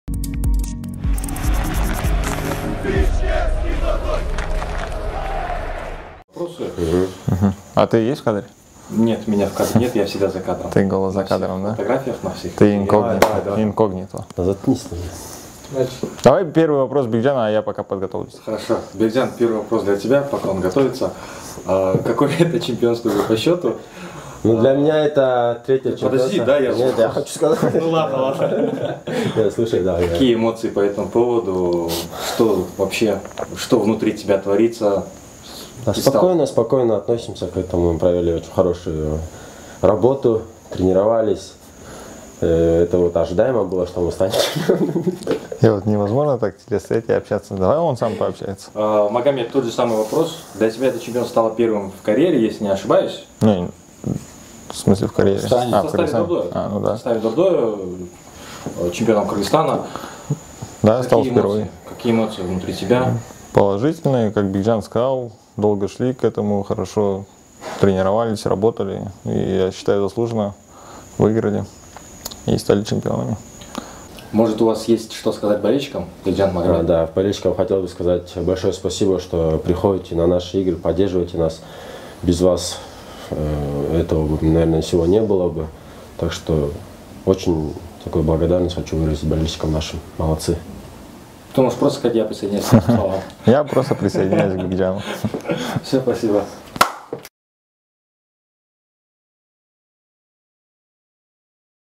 Maga & Bekja
Магамед Узденов и Бекжан Сагынбаев ответили на вопросы клубной пресс-службы